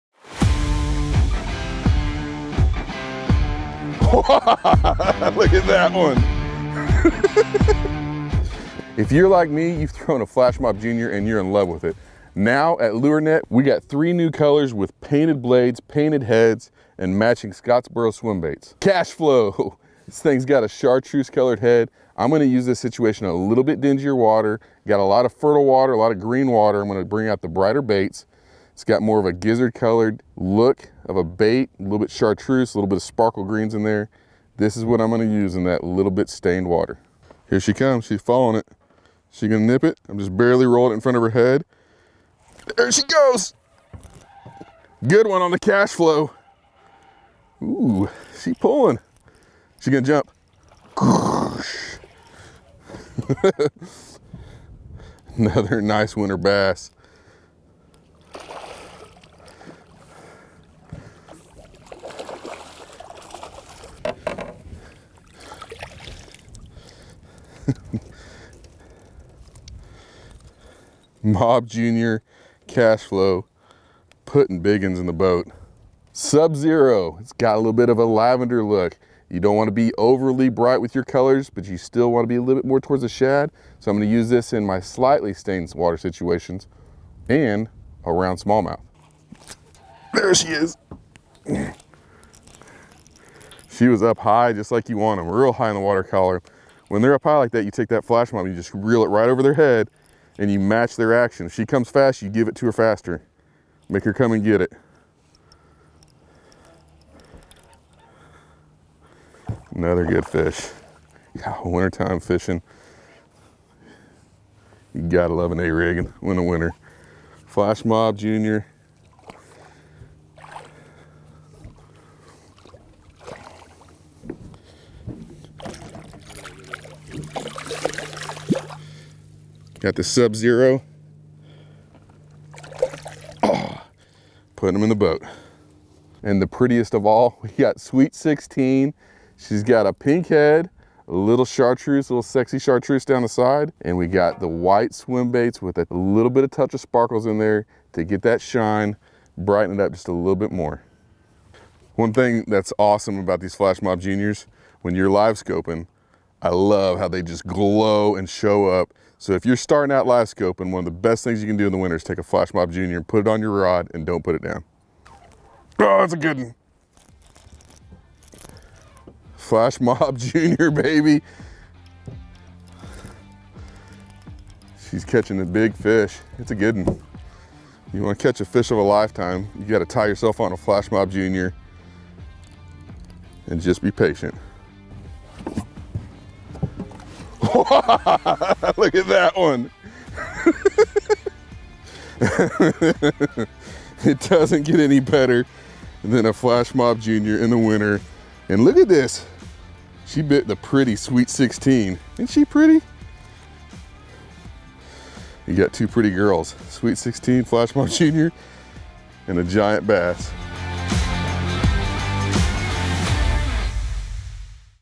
Custom Umbrella Rigs: The Secret to Catching GIANT Winter Bass (with John Soukup)
Custom umbrella rigs might be your secret to catching GIANT winter bass! Follow John Soukup as breaks down YUM's new custom Flash Mob Jr. and Scottsboro Swimbait kit colors and when to use them for winter bass fishing.